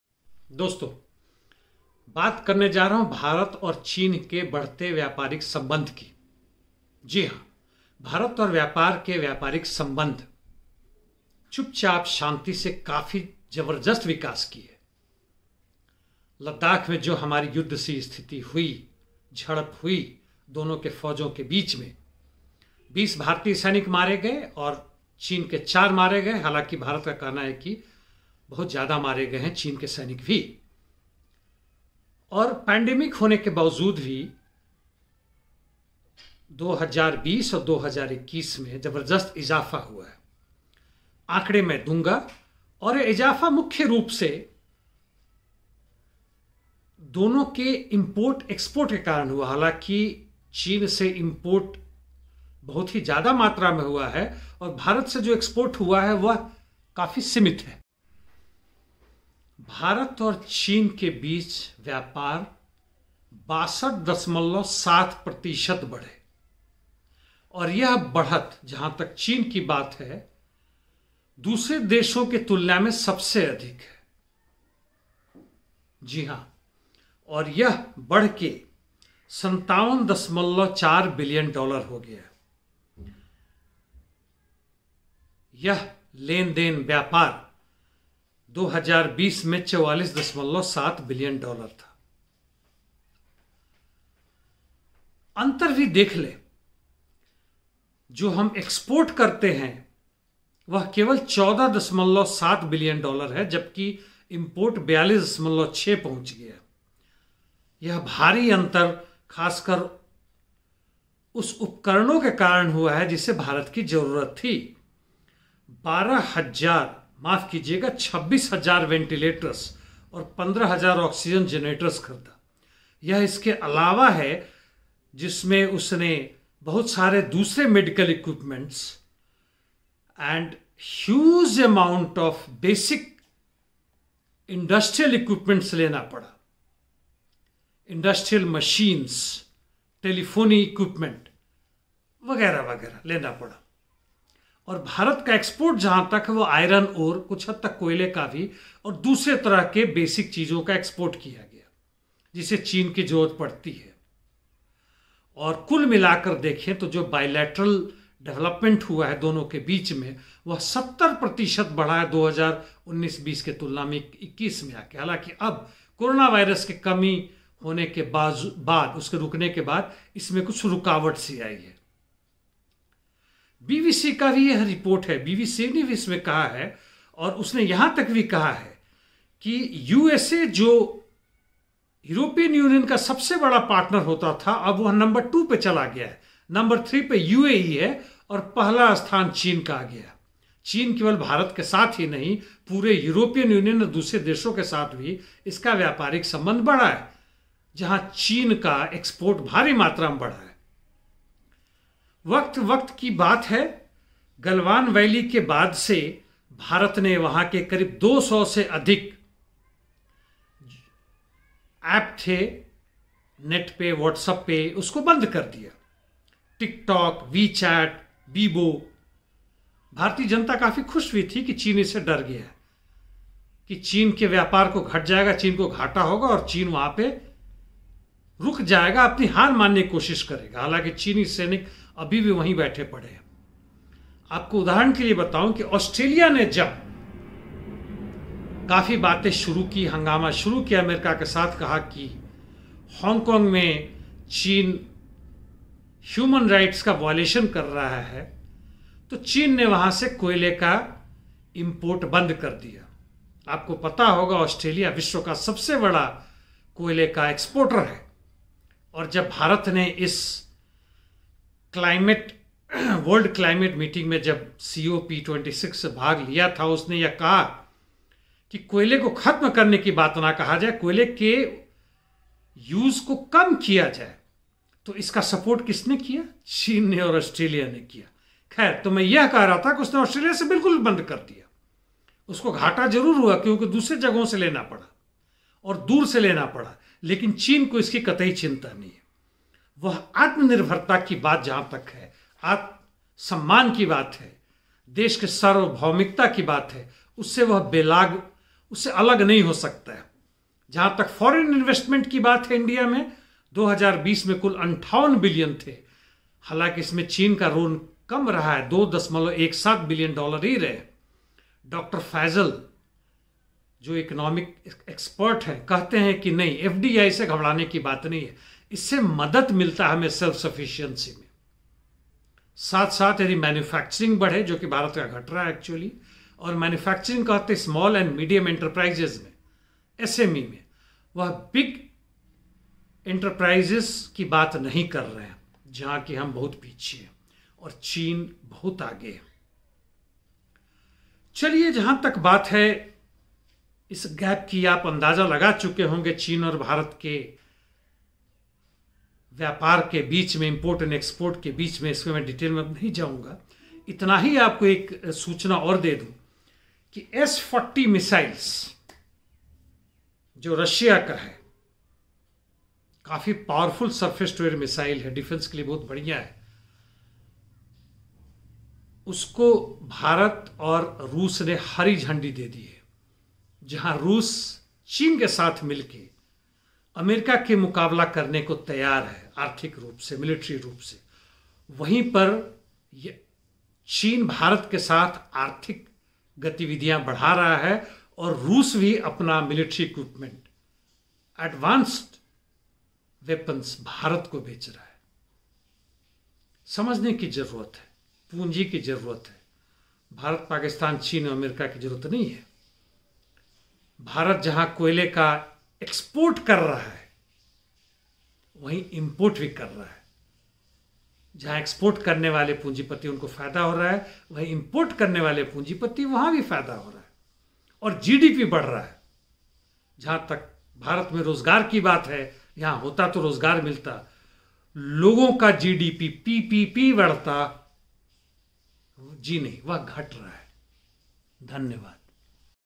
India China trade: Rising astronomically
China India trade is soaring despite boarder skirmishes